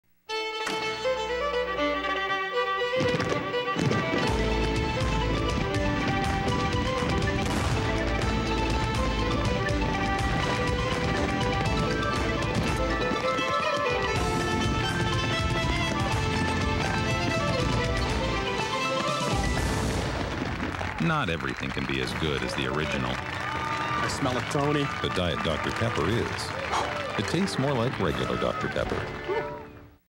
Diet Dr. Pepper Hudson Riverdance (commercial, 2001)
Dr. Pepper stages a showing of the fictional "Hudson Riverdance" featuring fat men wearing tank tops and kilts dancing in a vaguely Gaelic manner. The takeaway is that not all spinoffs can be as good as the original. But Diet Dr. Pepper is, since it tastes most like the original (non-Diet) version (according to Dr. Pepper).

Audience reaction to the show is mixed, with the man in the commercial thinking this is destined for a Tony Award while his better half isn't convinced.

This spot aired on FOX during a showing of Malcolm in the Middle on Sunday January 14th, 2001.